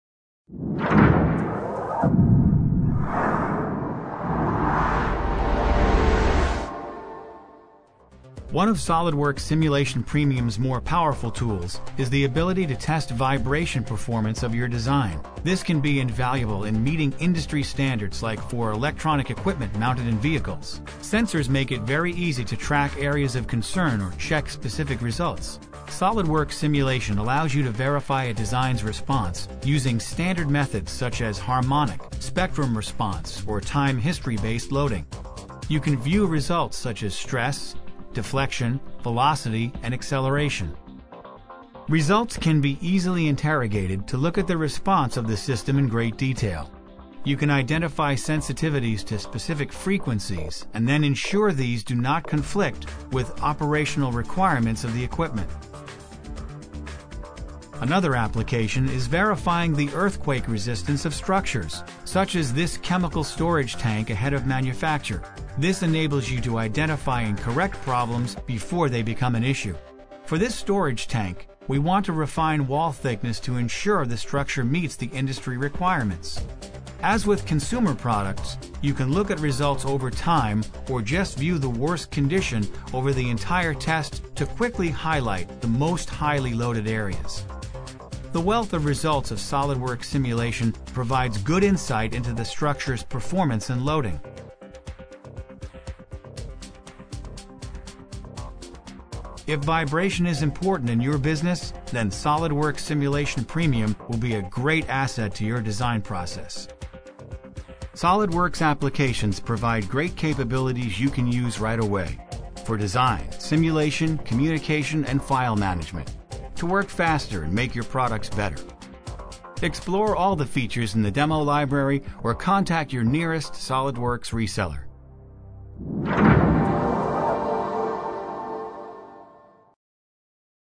Solidworks: Análisis de Frecuencias y Vibraciones
Investigue de forma rápida y eficaz las frecuencias naturales de un diseño (con cargas y condiciones de contorno y sin ellas) con la solución fácil de utilizar SOLIDWORKS Simulation. Asegúrese de que los modos naturales de vibración no se encuentran en las frecuencias de impulsos medioambientales, lo que indica que el diseño cumplirá la vida útil requerida.
y Distribuidor Autorizado Solidworks para Colombia